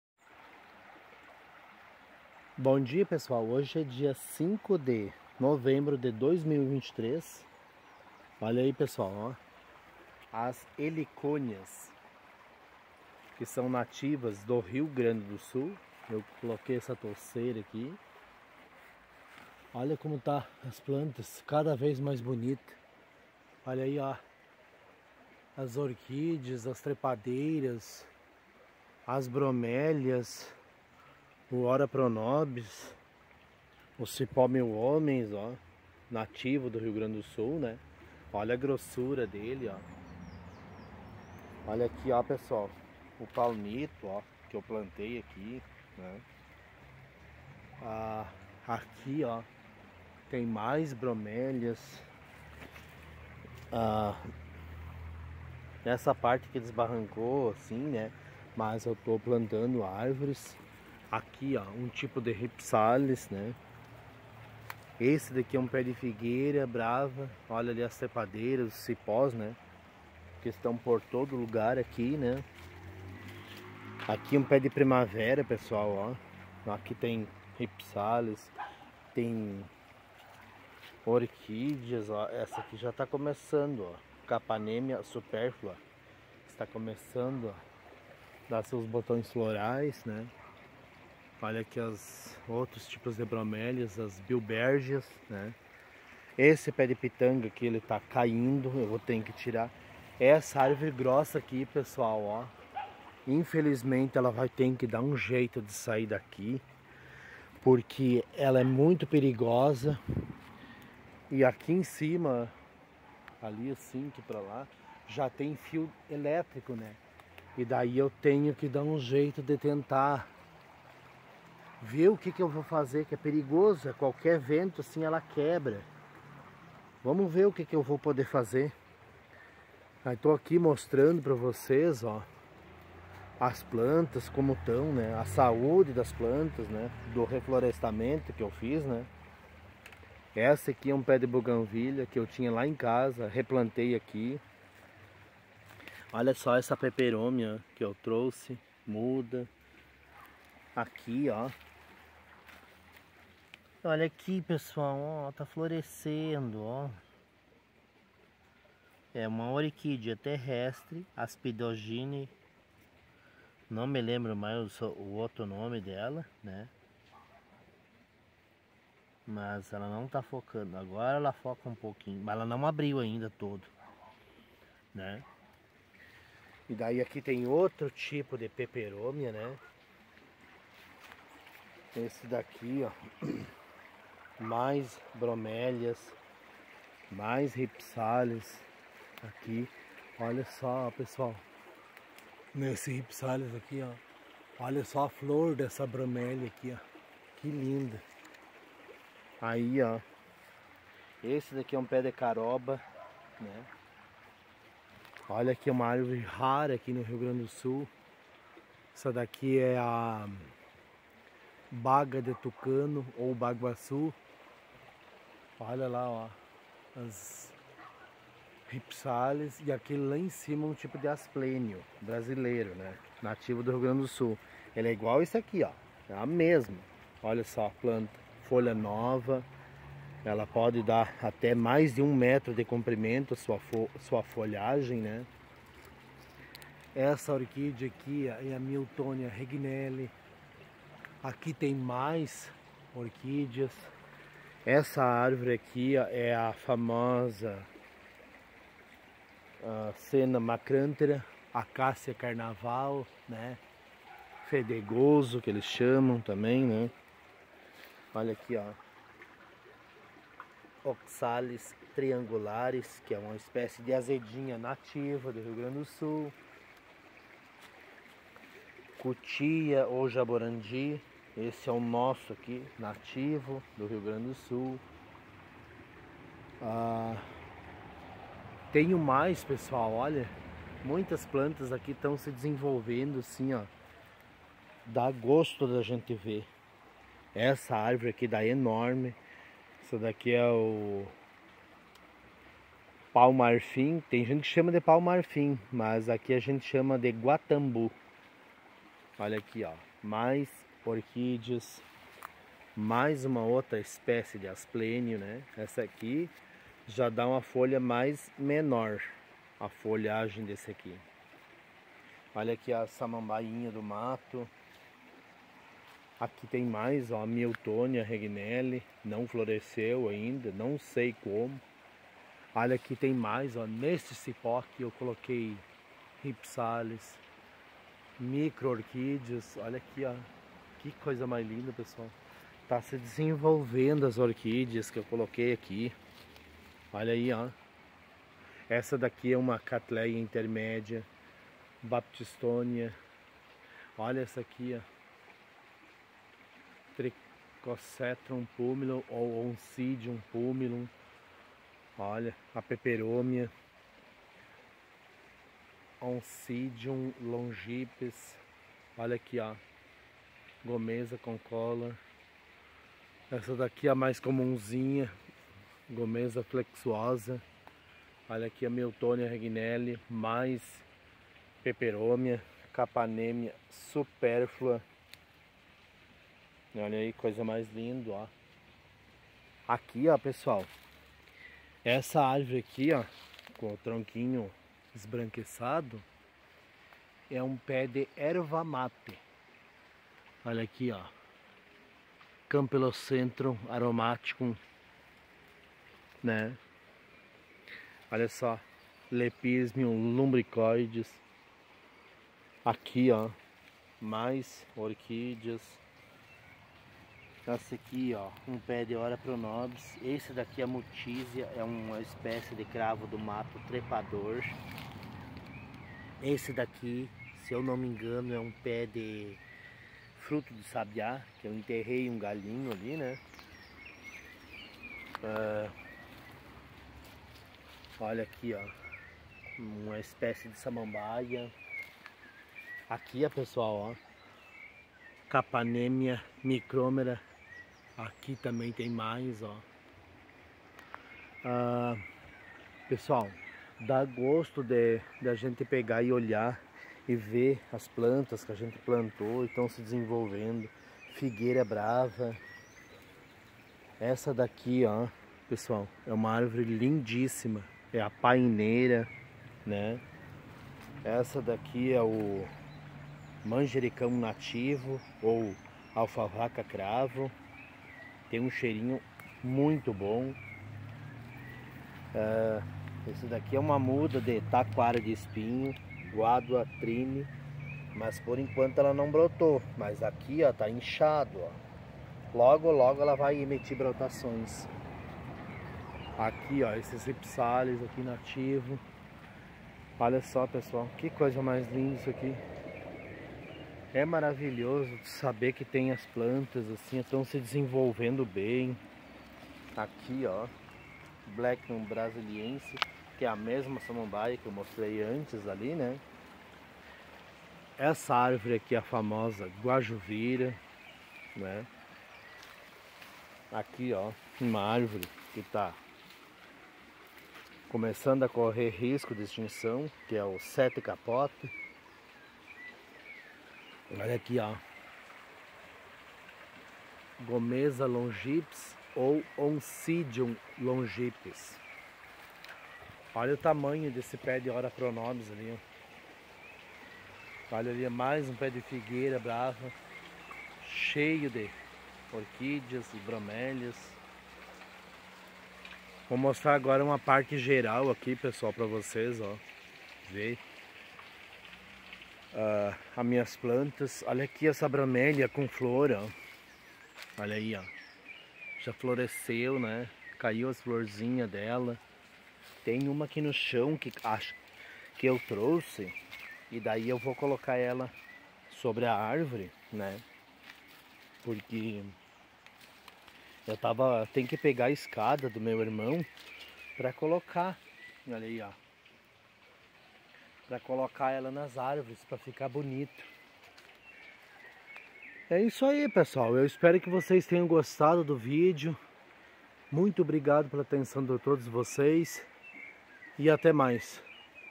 As plantas que tem no reflorestamento
Dia 05 de novembro de 2023( domingo)
Vim mostrar um pouco das plantas que tem aqui no reflorestamento.